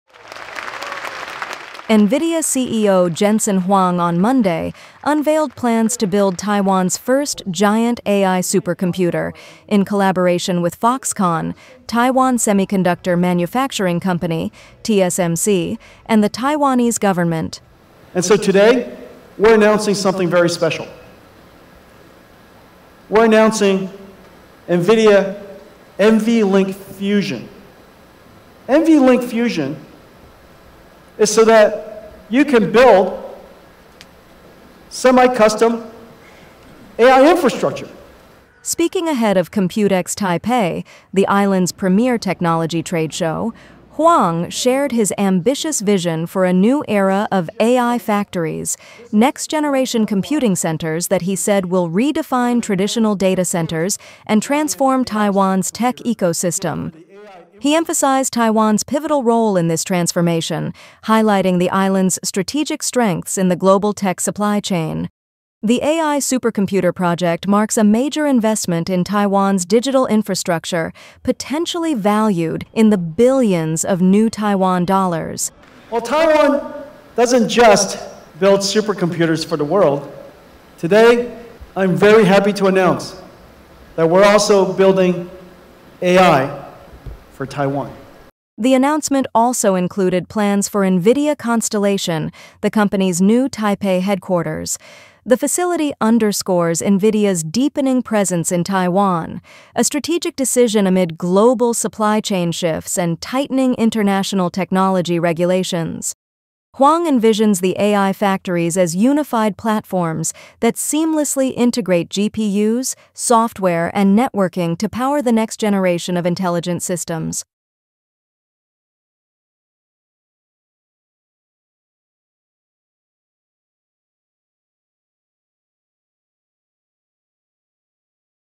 Nvidia's Huang unveils AI factory vision in Taiwan
NVIDIA CEO Jensen Huang has announced plans to build Taiwan’s first large-scale AI supercomputer, partnering with Foxconn, TSMC, and the Taiwanese government.

The initiative is part of Huang’s broader vision to establish “AI factories” — advanced computing hubs set to replace traditional data centers and solidify Taiwan’s strategic role in the global tech ecosystem.